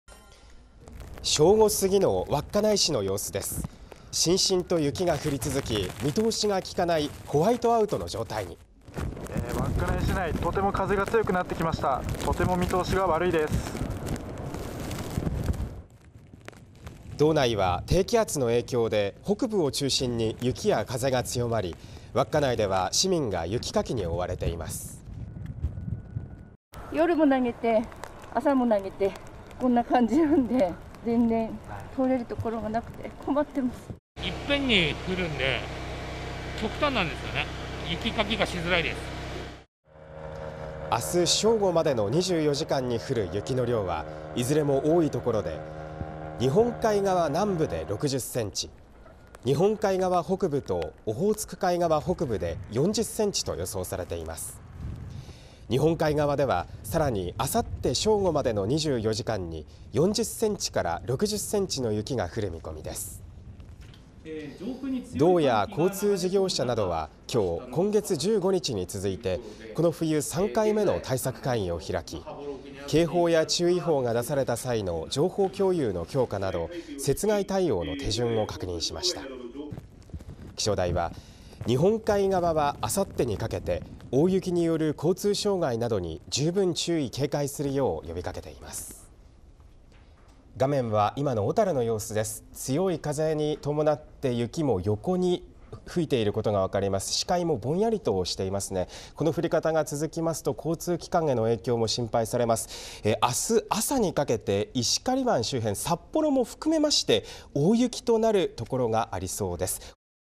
「とても見通しが悪い」風雪強く視界不良 週末にかけ大雪や交通障害に警戒 北海道
北海道は低気圧の影響で北部を中心に雪や風が強まっています。日本海側は23日にかけて大雪による交通障害などに警戒が必要です。(2023年12月21日放送)